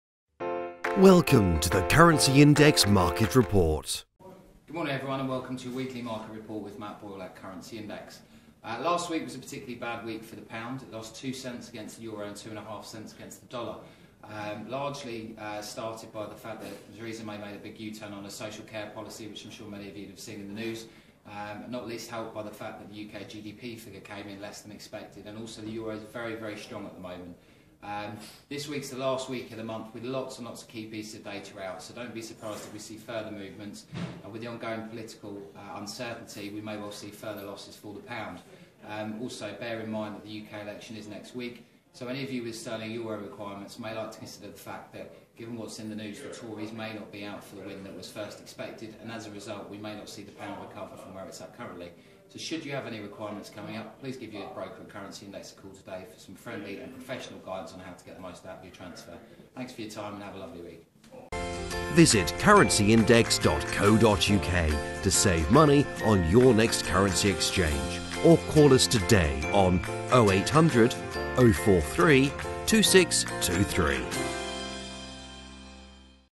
CINews Episode 180: Pound down ahead of UK elections
Last week was a bad week for the pound as it lost ground across the board as we get closer to the election on June 8th. General elections always breed uncertainty which is bad for a currency, however, political events are also not helping currently. Following Teresa Mays U-turn on her social care policy t seems the Conservative party are not as far ahead as first thought – something that was until recently pushing strength to the pound. Fears over a potential hung parliament are increasing as a mid-week poll showed Labour have closed the lead to 5 points behind the Tory party which is causing Sterling weakness over Brexit.